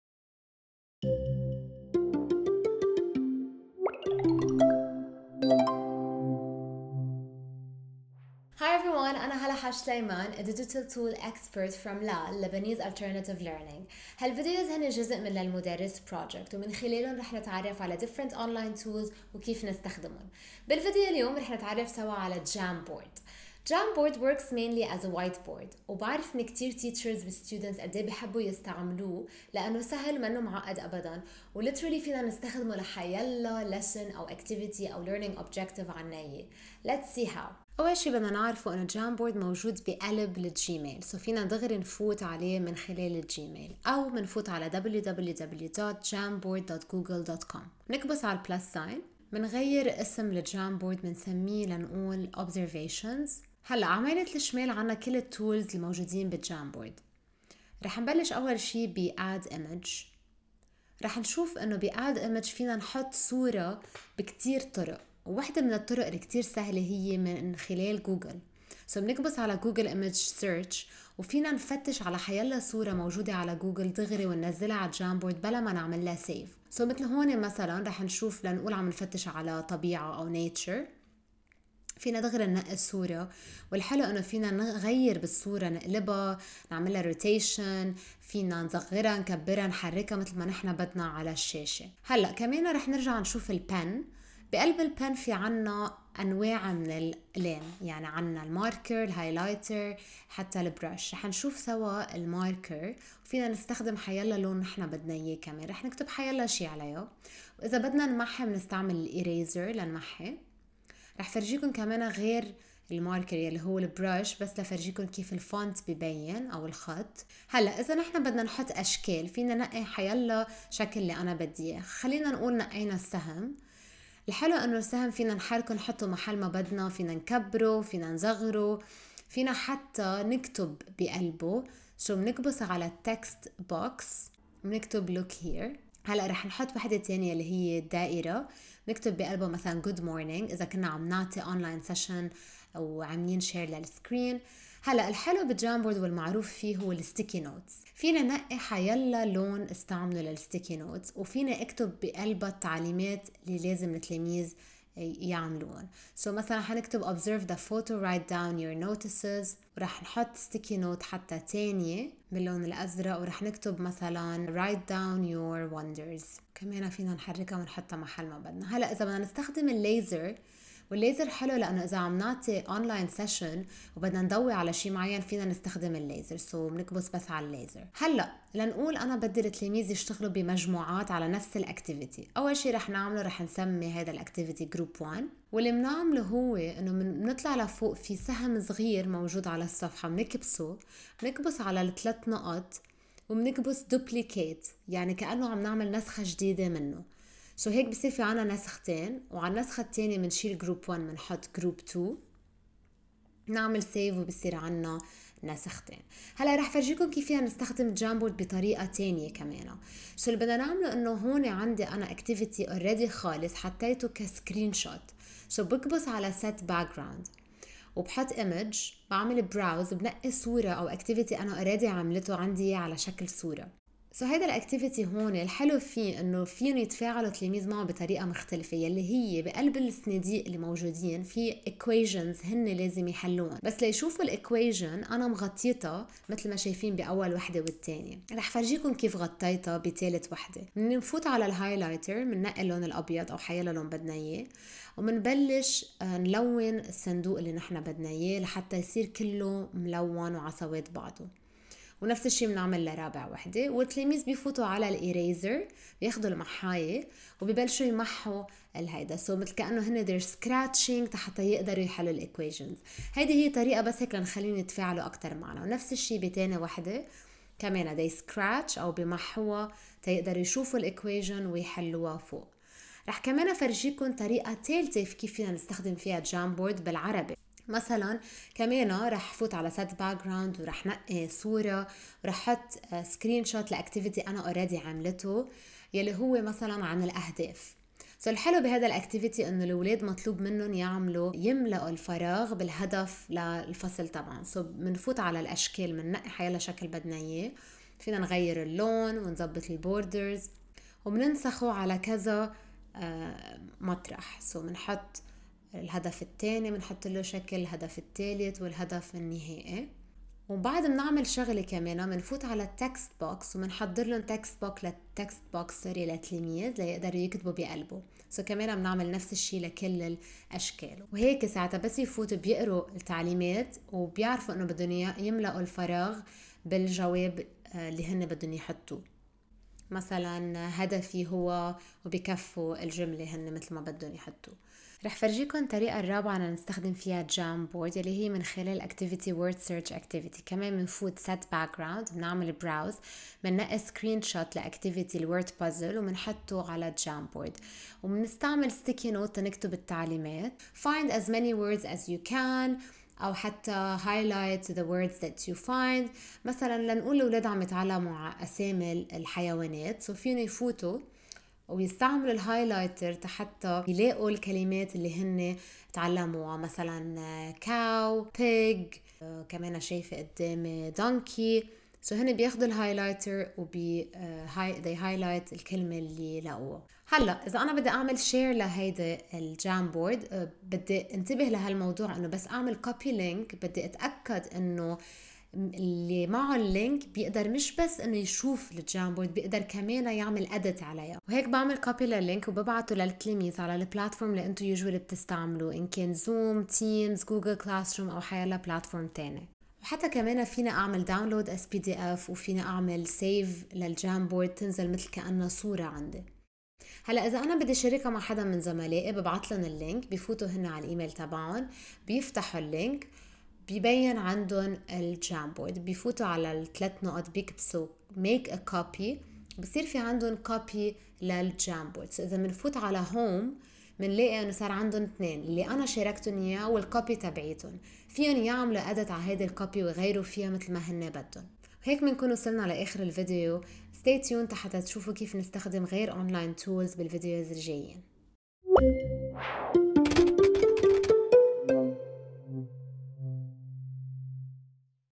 How to use Jamboard
Jamboard is a free interactive and collaborative whiteboard that’s connected to your gmail. Jamboard can be used as a canvas and a space for learners to collaborate together in real-time whether virtual or face-to face in class.